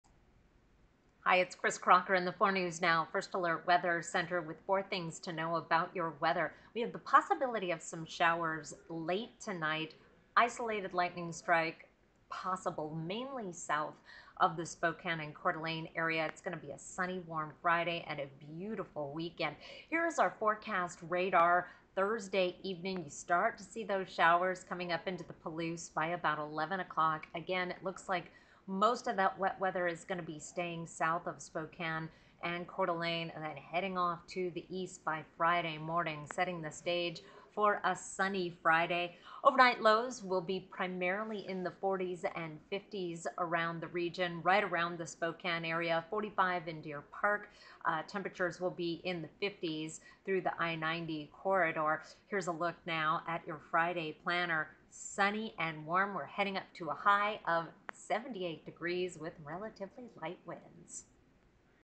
🌩Let the dog out early this evening: slight chance of showers or thunderstorms around 11 p.m. -
4 News Now Chief Meteorologist Kris Crocker has your First Alert Weather forecast!